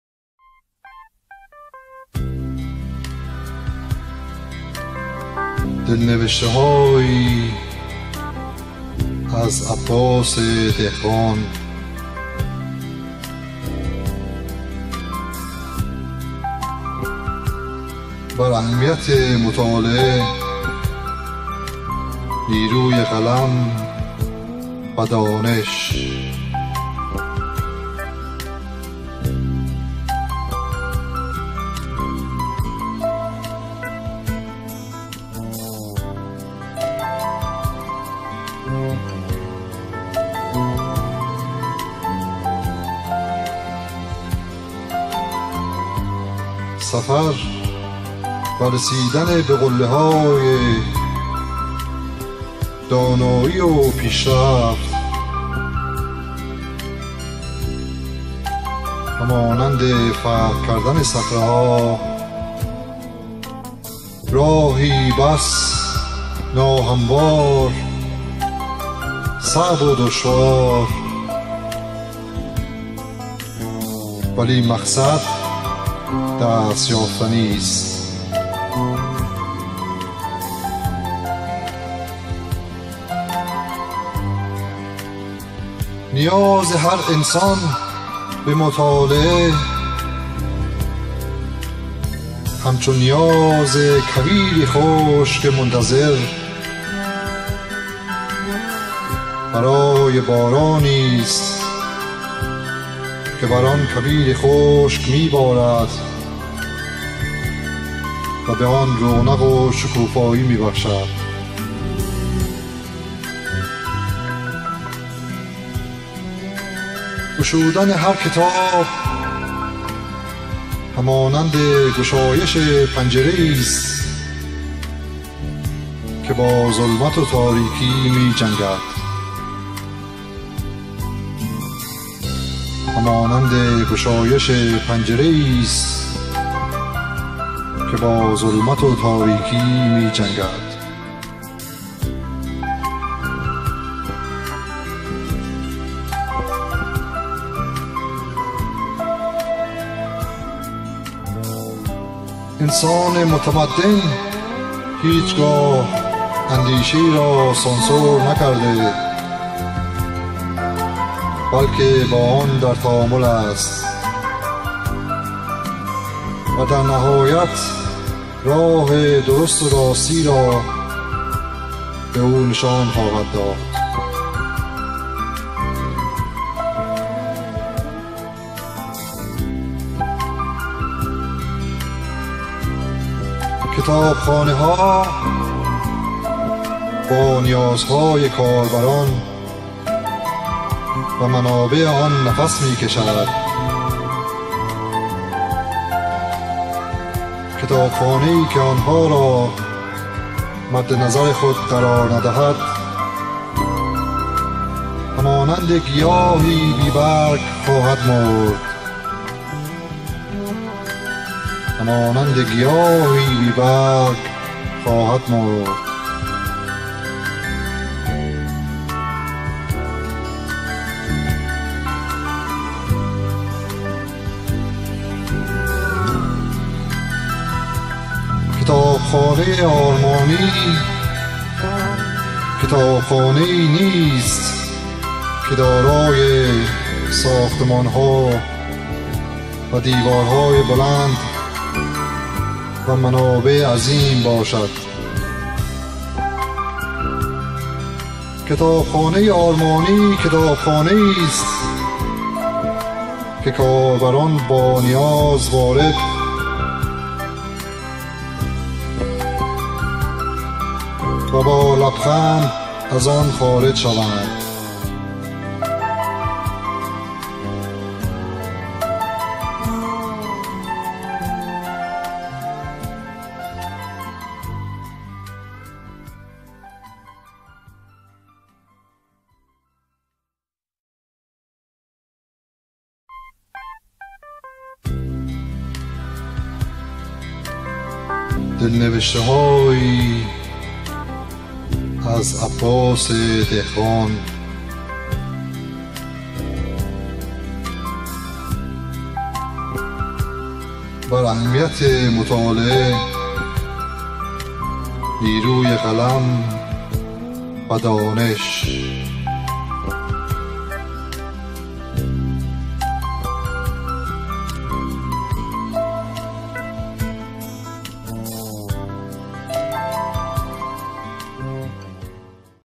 دلنوشته هایی از عباس دهقان بر اهمیت مطالعه، نیروی قلم🖊 و دانش📚
دلنوشته هایی از عباس دهقان

بر اهمیت مطالعه، نیروی قلم🖊 و دانش📚

اگه ایرانی هستید و مطالب ارائه شده در این اثر را مفید دانستید حتما با دوستانتان به اشتراک بگذارید

با سپاس فراوان عباس دهقان ❤🌺